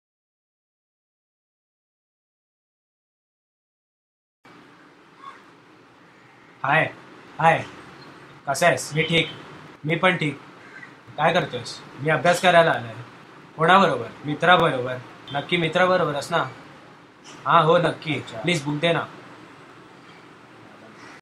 Double Role of Akshay
Just tried making a double role with my cousin